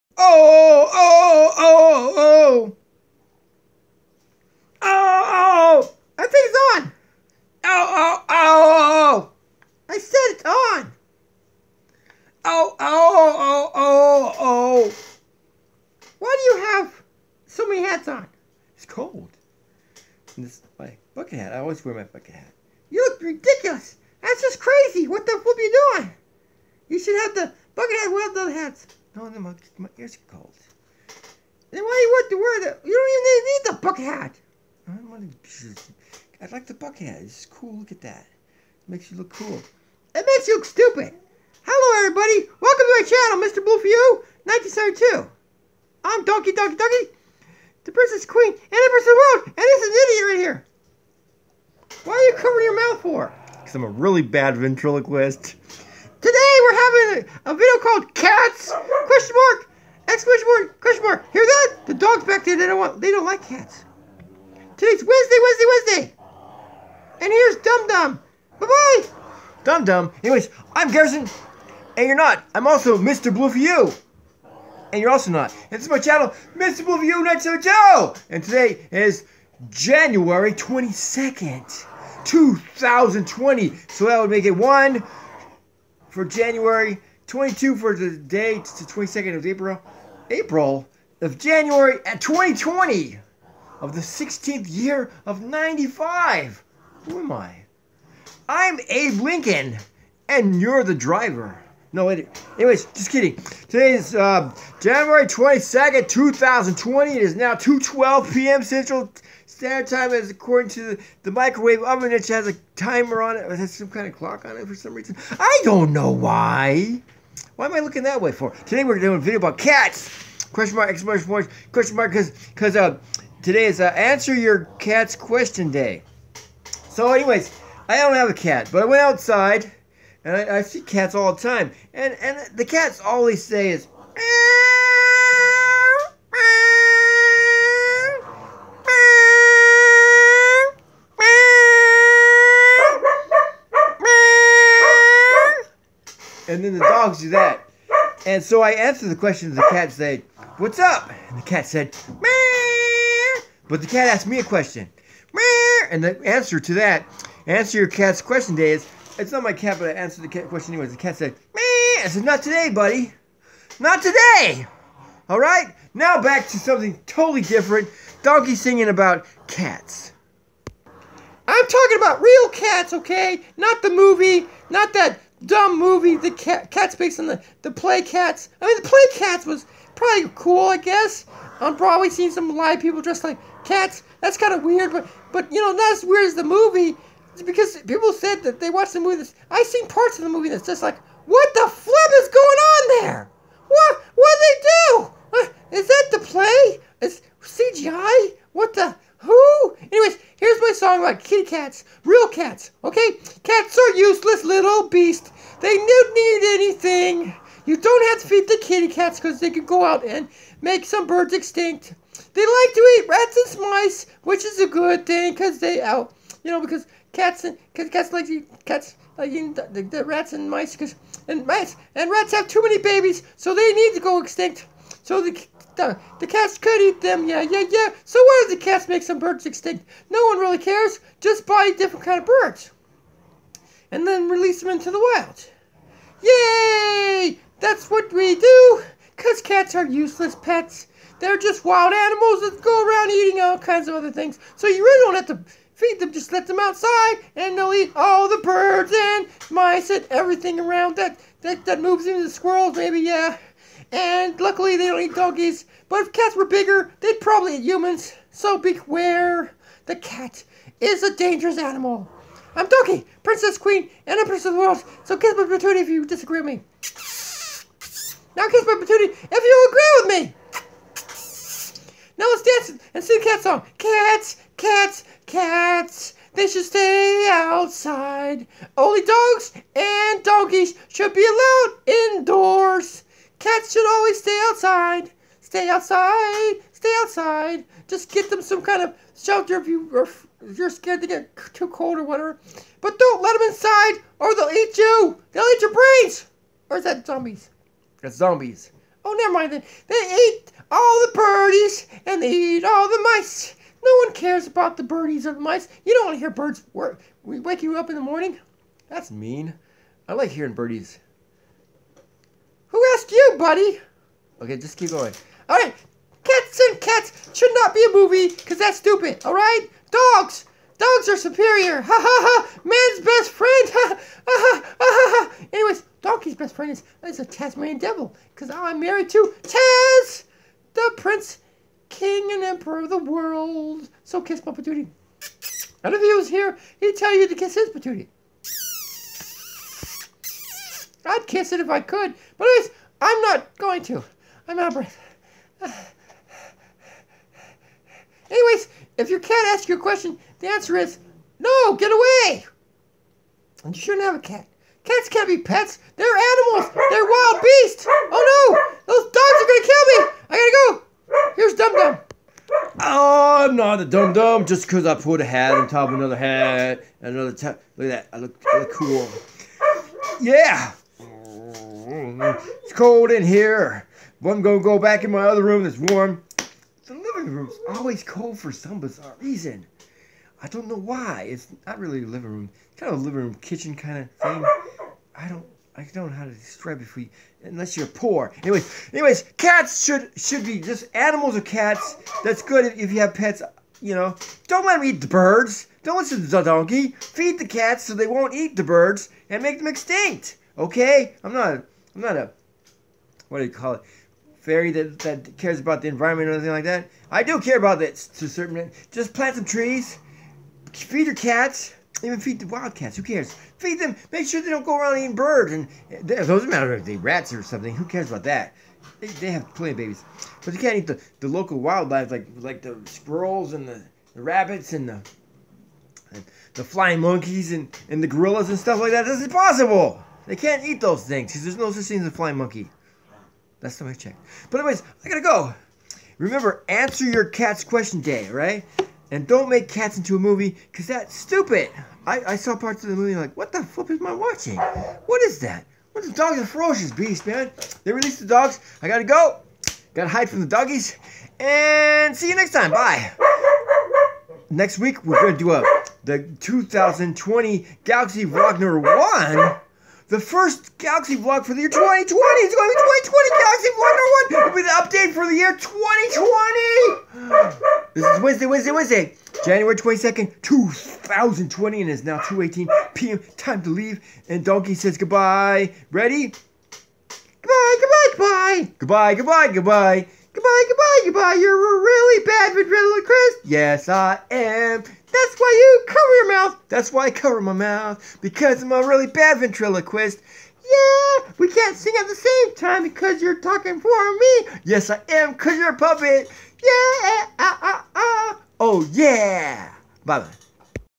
Cats?!?
In this strange video, I, Garrison, along with Donkey, discuss and talk about "cats" and other unrelated things! I really hope you like this video! Behave! (Watch out for cats! They're everywhere!)

(Done on my Galaxy Tab A 9.7, in the downstairs kitchen, sitting on a barstool with a puppet.)